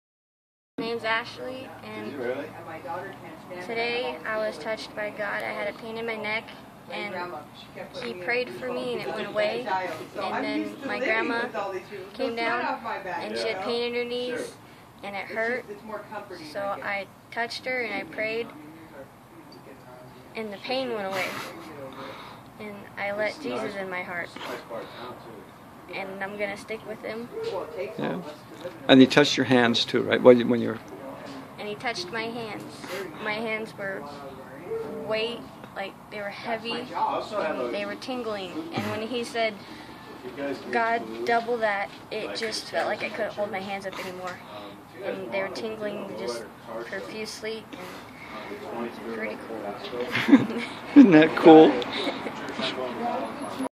Ashley - Healed Then Saved
A friend of mine, Mike, asked me to go with him to pray for a women dying from a brain tumor.  We ended seeing two other family members healed and this young lady asked Jesus to be her Lord.  She was healed, then she saw her grandmother healed through her hands, and then she invited Jesus to save her.

Revival is here!